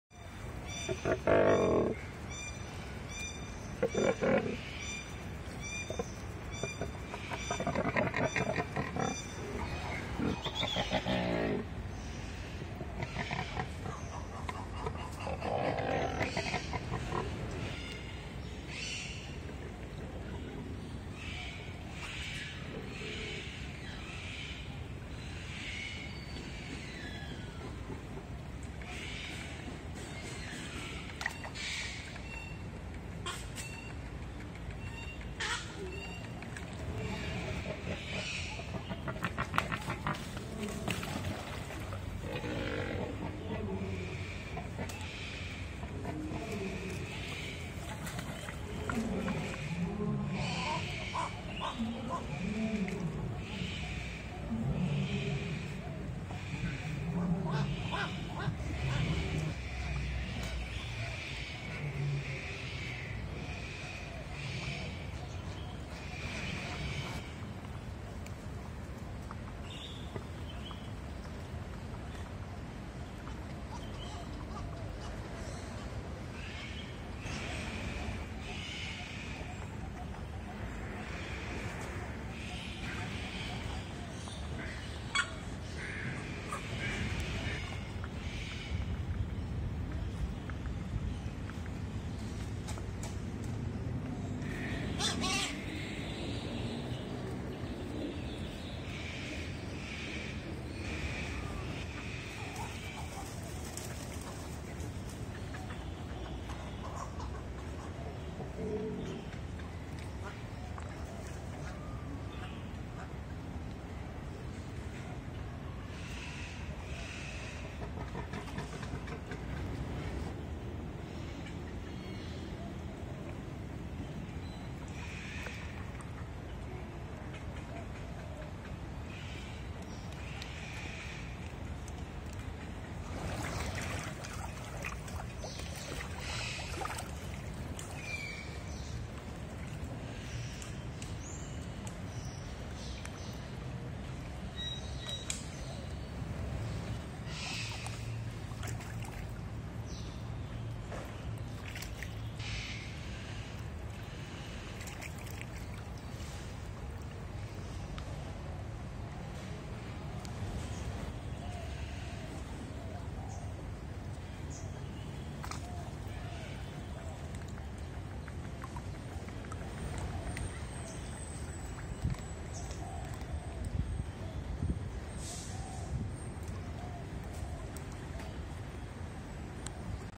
Tes Kamera ASUS Zenfone 3 (ZE520KL) - Australian Pelican Di Kebun Binatang Bandung
Melihat Australian Pelican berenang di Kebun Binatang Bandung, video direkam menggunakan smartphone ASUS Zenfone 3 (ZE520KL).